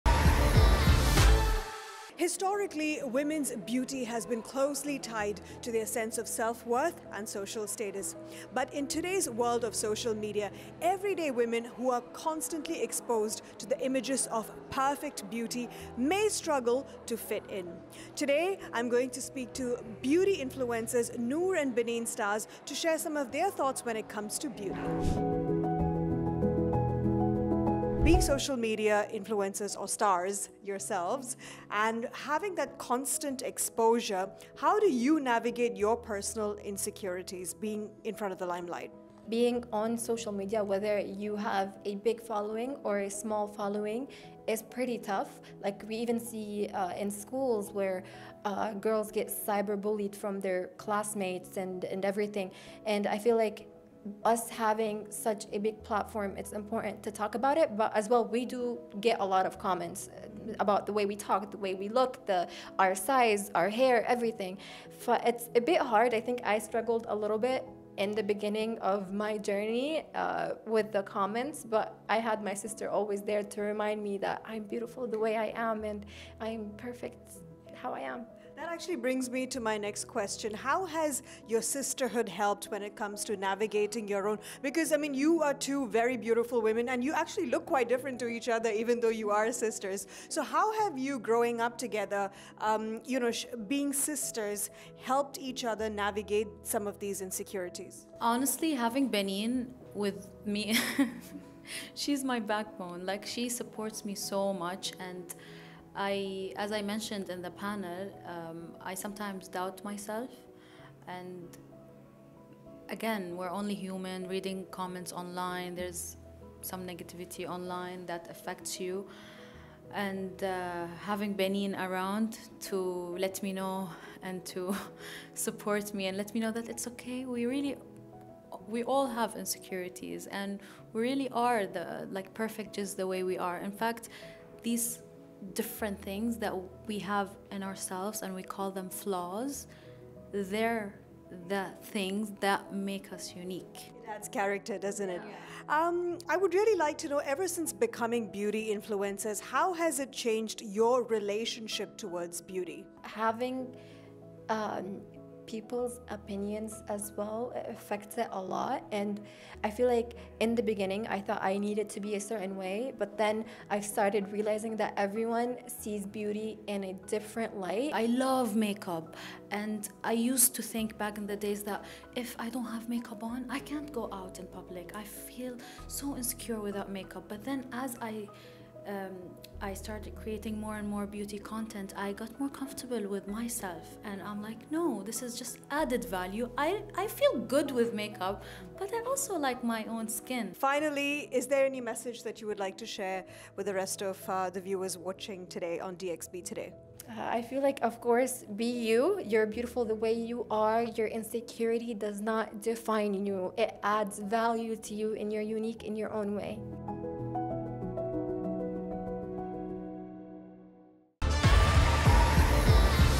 Noor and Baneen Stars: The Influencer Sisters Changing The Beauty Game | Exclusive
In an exclusive meet-up with beauty sisters Noor and Baneen Stars, we dive into the unfiltered truths of being an influencer. They share their experiences, challenges, and the pressure of maintaining an online persona, offering insights that go beyond the glam. Join us as we explore what it's really like to navigate the world of beauty and social media in today's fast-paced digital landscape.